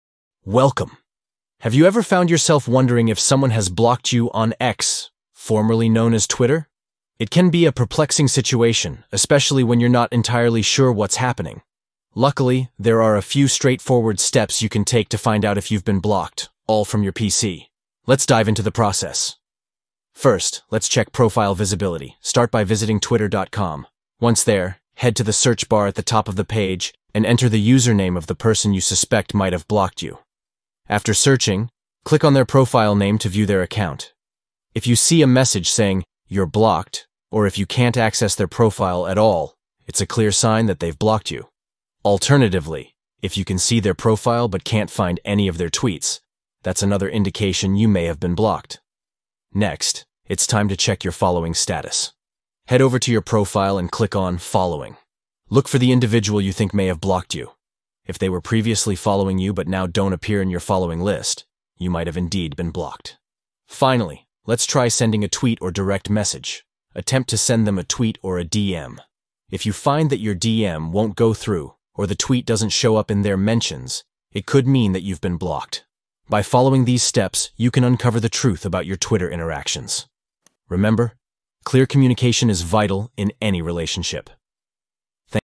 How To Know If Someone Blocked You On X (Twitter) On PC (New Update)
Learn how to find out if someone has blocked you on X, also known as Twitter, with simple steps on your PC. Discover the signs that indicate you may have been blocked by checking profile visibility, your following status, and trying to send a message.

Steps:
1. Check Profile Visibility
2. Check Your Following Status
3. Send a Tweet or DM

Chapters:
00:00 Intro
00:12 Soultion
01:05 Outro